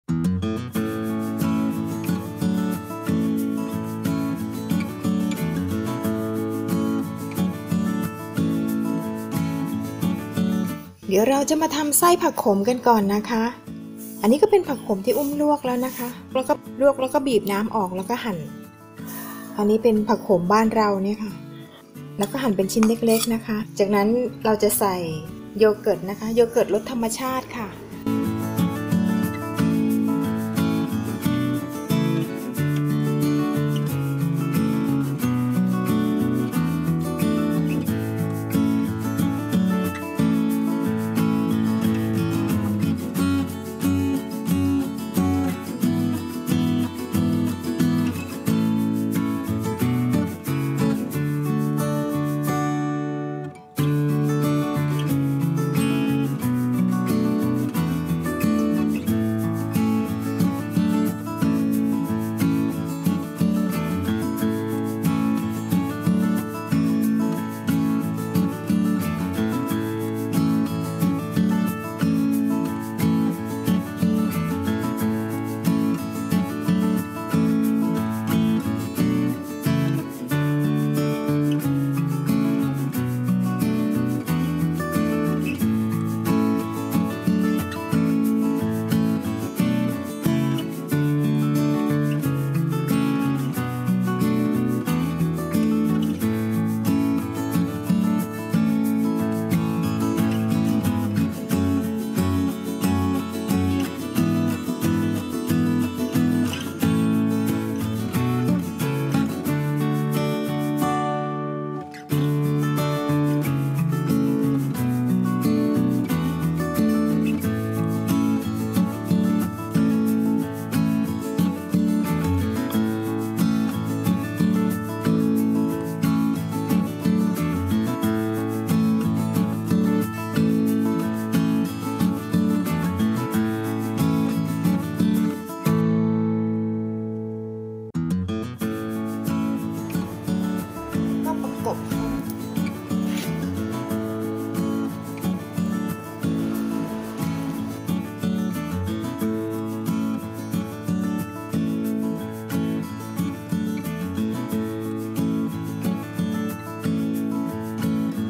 อาหารจานโปรด EP. 32 เมนูโปรด แต้ว ณฐพร ขนมปัง ขนมปังชีสผักโขม แซนวิชชีสผักโขมสูตรโยเกิร์ต
ขนมปังชีสผักโขม เป็นขนมของโปรด แต้ว ณฐพร ค่ะ วันนี้มีสูตรขนมปังแซนวิชชีสผักโขมสูตรโยเกิร์ต อร่อยๆ มาฝากเพื่อนๆ นะคะ ทำง่ายมากๆ แต่พอดีวันนี้ใช้ขนมปังค่อนข้างหนามาก กว่าชีสจะเริ่มละลาย ขนมปังเลยค่อนข้างกรอบไปสักหน่อยแต่ไม่เป็นไรค่ะ ไม่ถึงกับไหม้ เพราะใช้ไฟอ่อนมากๆ ค่อยๆ ย่าง แบบนี้ก็ดีค่ะ กรอบนอก นุ่มใน หอมกลิ่นหนมปังปิ้งมากๆ เพราะค่อนข้างเกรียม^^